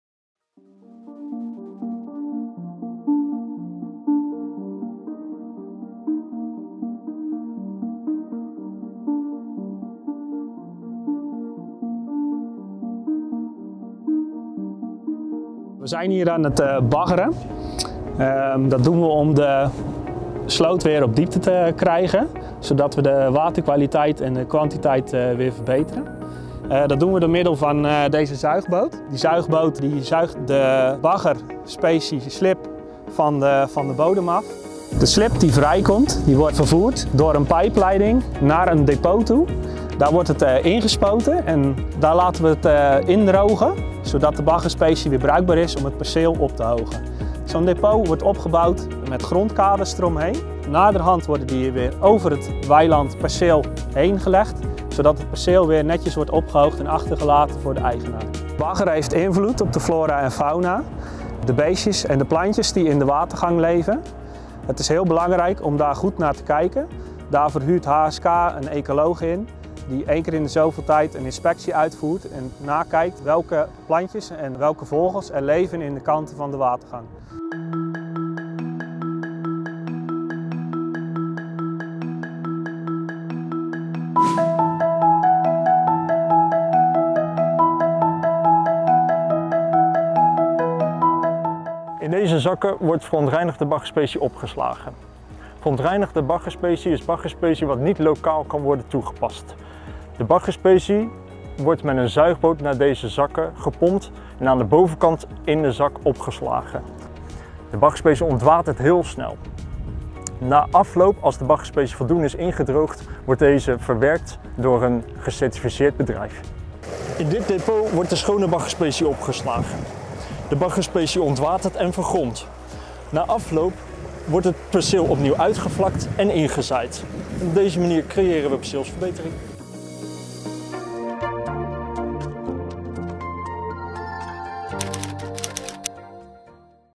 Baggeren in de Krimpenerwaard met baggerboot
door het hoogheemraadschap van Schieland en de Krimpenerwaard